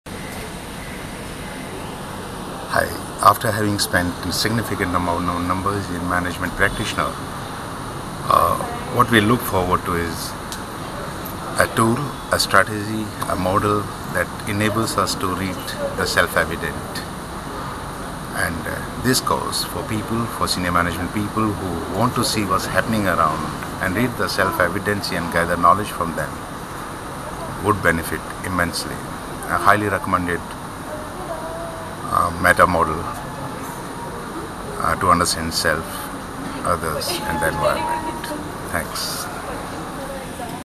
NLP Master Practitioner in Goa 2015 - Ashok
NLP Master Practitioner in Goa 2015 - Ashok Bhatotia talks about his experiences.